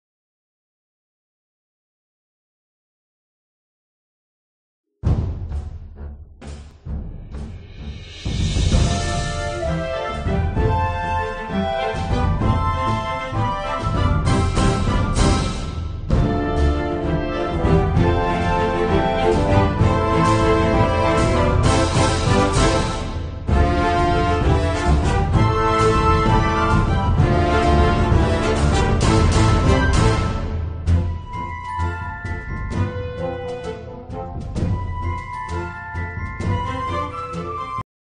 Which fraction is biggest for vidumath Norway
Which fraction is biggest by students of Class 6c at Hallset skole in Trondheim, Norway